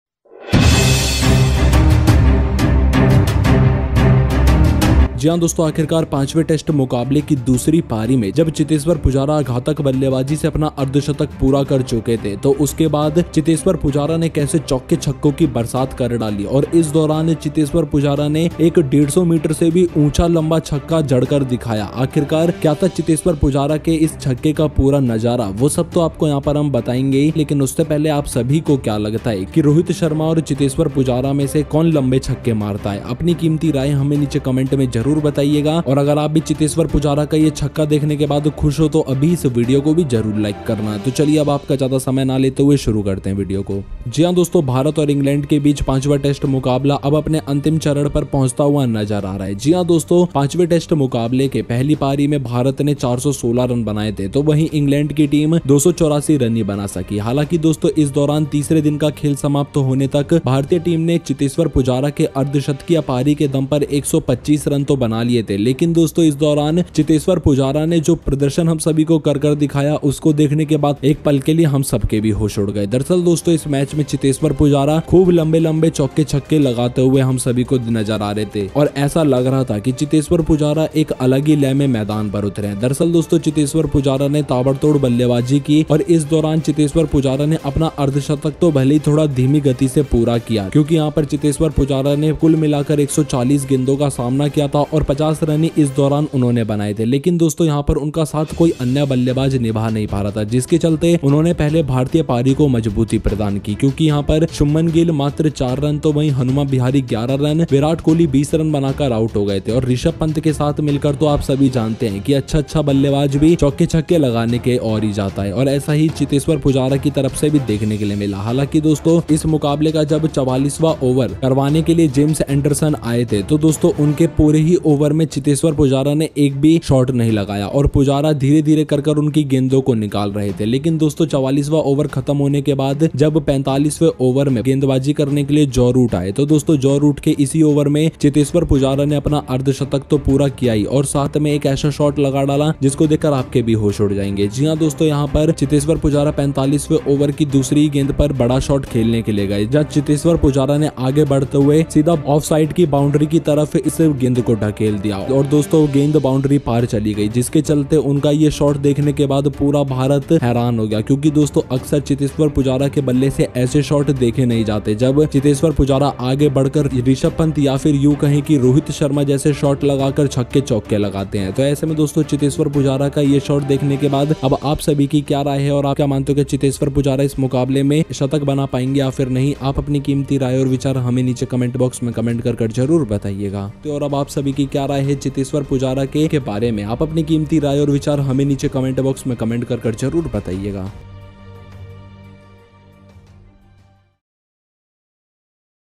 देखिए,तीसरे दिन Pujara ने मैच के दौरान गुस्से में जड़ा 150M लंबा छक्का,देखकर Pant,Kohli के भी उड़े होश
ABOUT CRICKET NEWS: Friends, the fifth test match between India and England now seems to be reaching its final stage. Yes, friends, India had scored four hundred and sixteen runs in the first innings of the fifth test match, while England's team could only score two hundred and eighty-four runs. Although friends, till the end of the third day's play, the Indian team had scored one hundred and twenty-five runs on the basis of Cheteshwar Pujara's half-century, but friends, during this time, Cheteshwar Pujara showed us all the performance. For a moment all of us were also blown away. Actually friends, in this match, Cheteshwar Pujara was seen hitting very long fours and sixes and it seemed that Cheteshwar Pujara landed on the field in a different rhythm. Actually friends Cheteshwar Pujara batted vigorously and during this Cheteshwar Pujara completed his half-century even though it was a bit slow because here Cheteshwar Pujara had faced a total of one hundred and forty balls and fifty runs were scored during this period. But friends, no other batsman was able to play with him here. Due to which he strengthened the first Indian innings because here Shubman Gill only four runs and Hanuma Vihari eleven runs. Virat Kohli was dismissed after scoring twenty runs and together with Rishabh Pant, you all know that even a good batsman goes for hitting fours and sixes and the same was seen from Cheteshwar Pujara's side. Although friends, when James Anderson came to get the forty-fourth over of this match, friends, Cheteshwar Pujara did not hit a single shot in his entire over and Pujara was slowly removing his balls. But friends, after the end of forty-four overs, when Joe Root came to bowl in forty-five overs, then Cheteshwar Pujara has completed his half-century in the same over of friends, and together he put a short, seeing which your senses were also blown away. Will go

Yes friends, Cheteshwar Pujara went here to play a big shot on the second ball of forty five overs. Cheteshwar Pujara, moving forward, pushed the ball straight towards the boundary of the offside and friends, the ball went across the boundary, due to which the whole of India was surprised after seeing his shot.

Because friends often do not see such shorts with Cheteshwar Pujara's bat, when Cheteshwar Pujara goes ahead and hits sixes and fours by hitting shots like Rishabh Pant or u say Rohit Sharma, then friends, after seeing Cheteshwar Pujara's 150m long shot here, all of you What is your opinion and what did you believe?

Cheteshwar Pujara will be able to score a century in this match or not, you will definitely tell us your valuable opinion and thoughts by commenting in the comment box below and what is your opinion now?